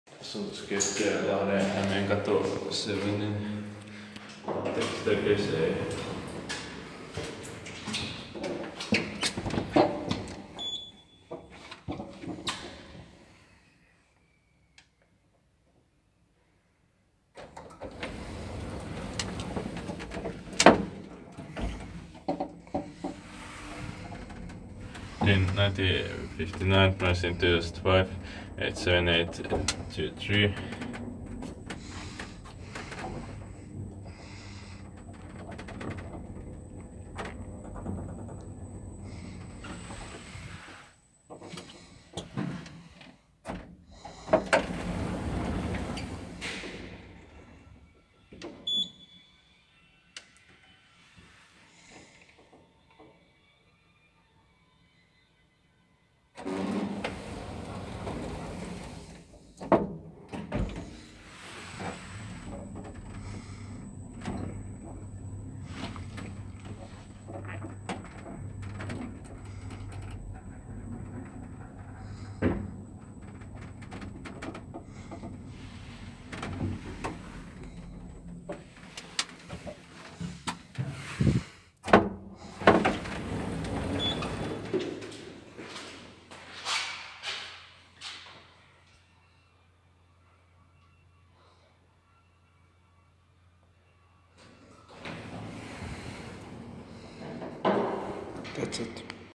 Nice 1959 KONE (Mb KONE 2005) Traction Elevator at Hämeenkatu 7A, Lahti, Finland
Location: Hämeenkatu 7A, Lahti, Finland
Brand: KONE
Year installed: 1959
Year modernized: 2005
Serial number: H 7823
Type: Traction
Floors: 5 (1*, 2, 3, 4, 5)
Fixtures: KONE Delta
Capacity: 3 persons or 240 kg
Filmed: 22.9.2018